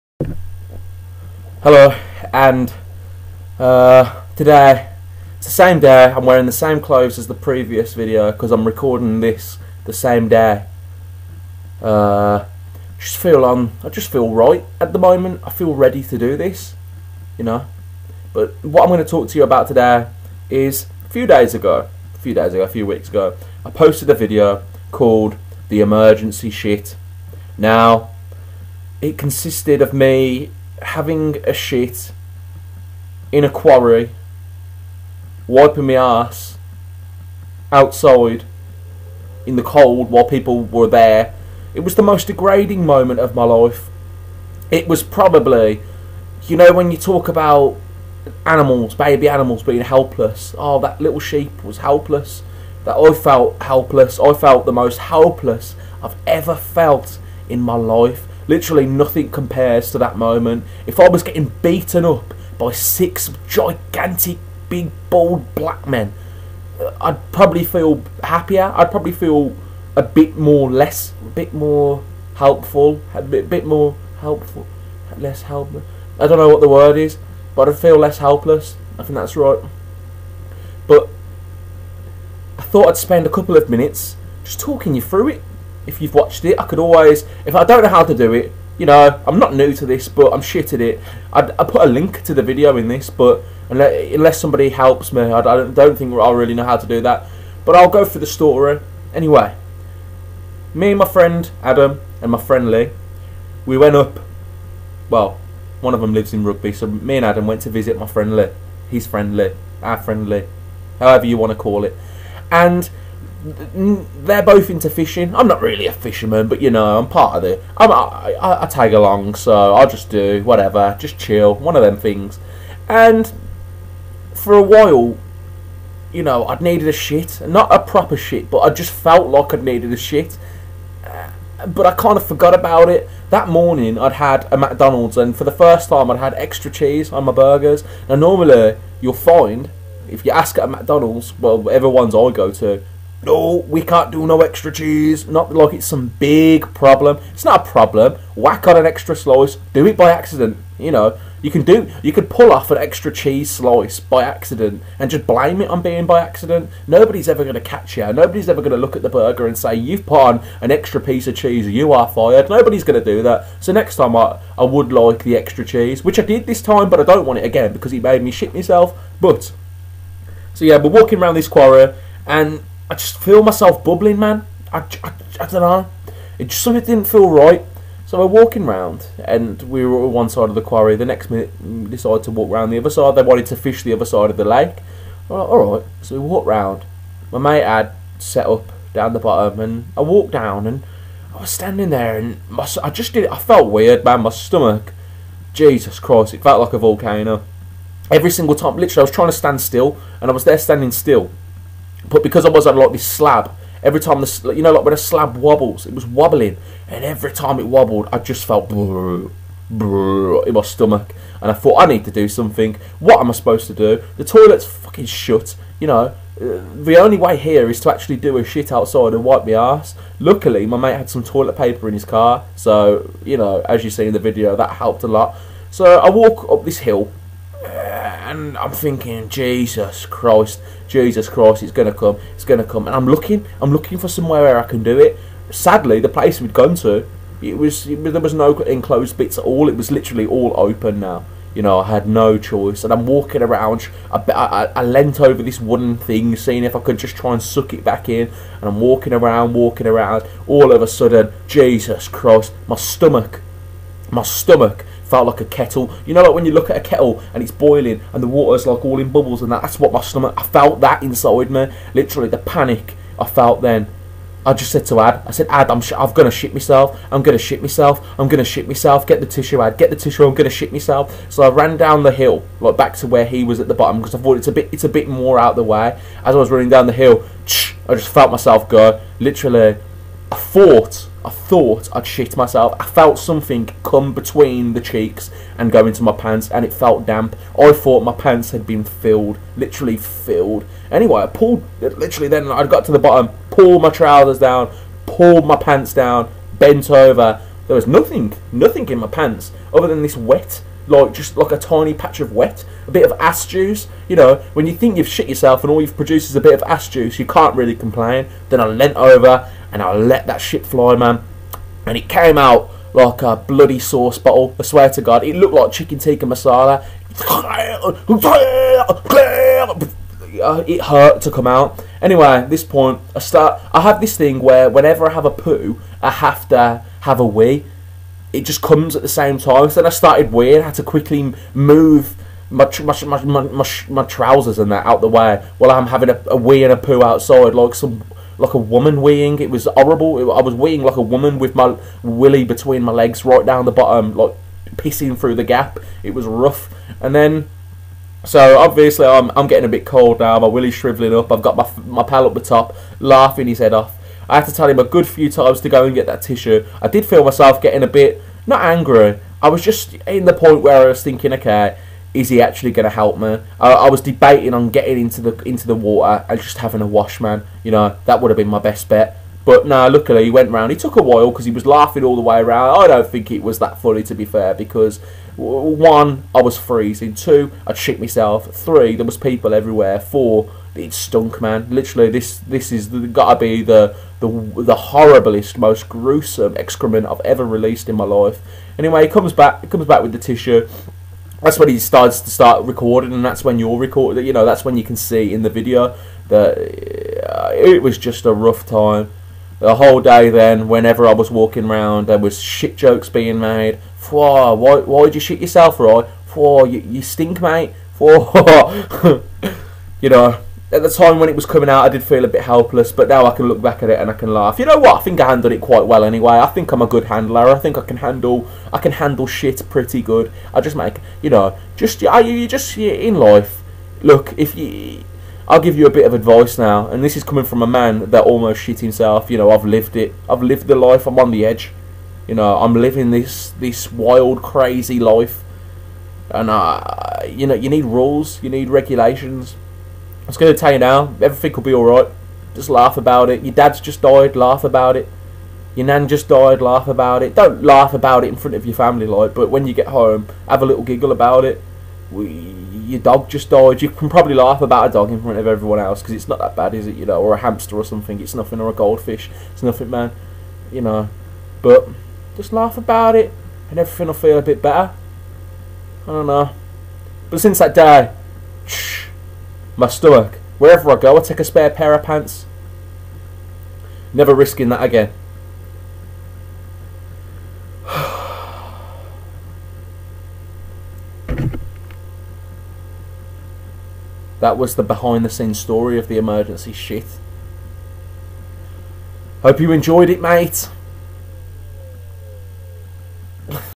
THE STORY BEHIND THE DAY I SHIT MY PANTS!
I thought I'd spend a couple of minutes explaining the time i shit my pants...